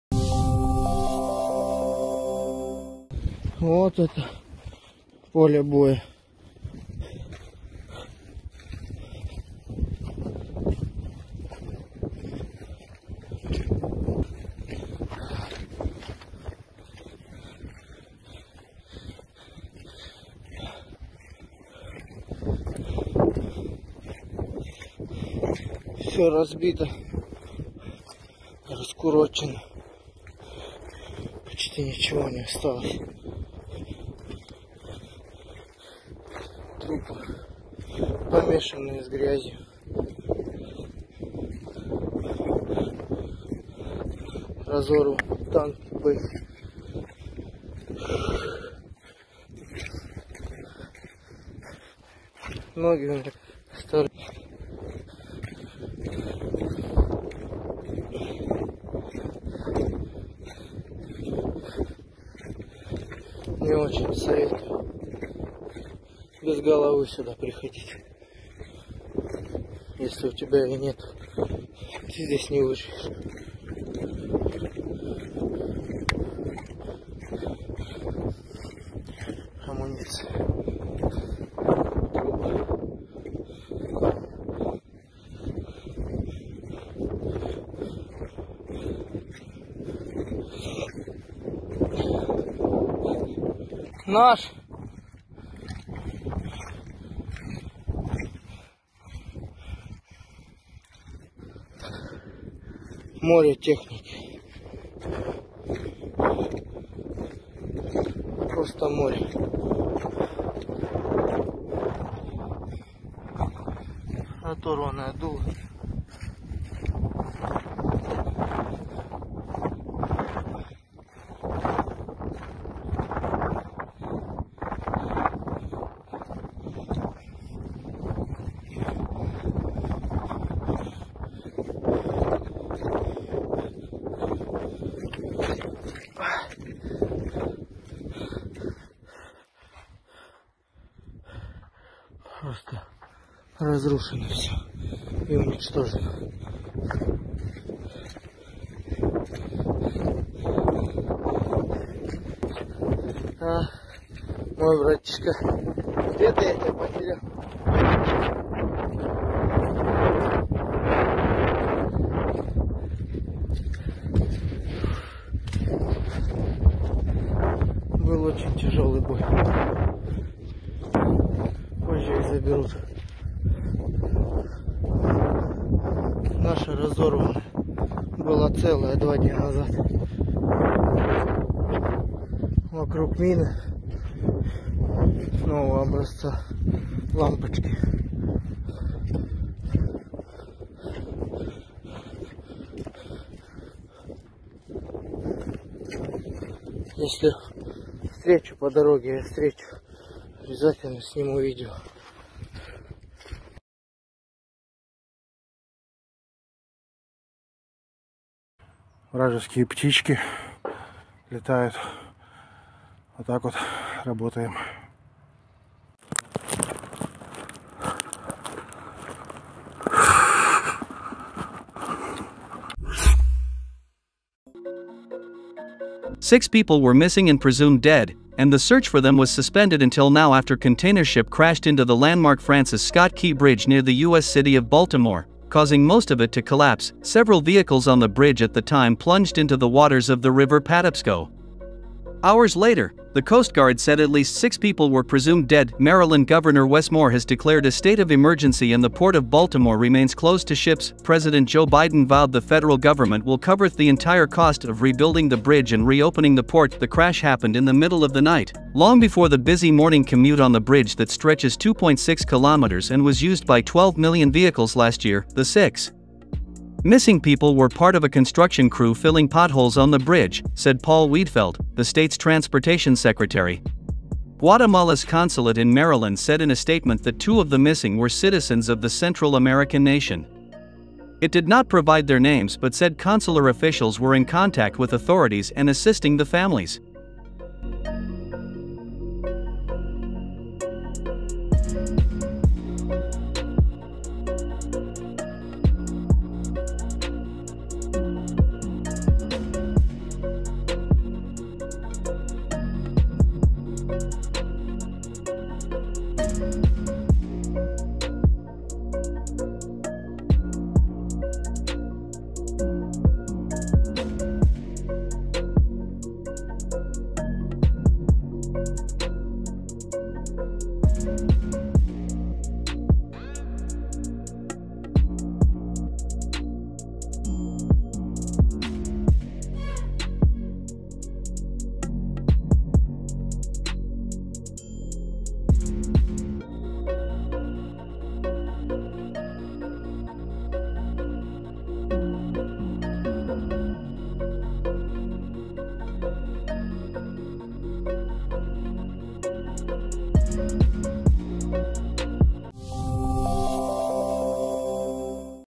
"The soldiers’ c*rpses are in mud" - Footage of abandoned Russian position
The soldier of the occupying Russian army left his position in the village of Robotino in the Zaropozhye region. The soldier demonstrated the remains of armored combat vehicles and Russian soldiers destroyed as a result of the strikes of the Ukrainian army. The occupying soldier said that the last few soldiers were able to leave the area and advised not to go to war.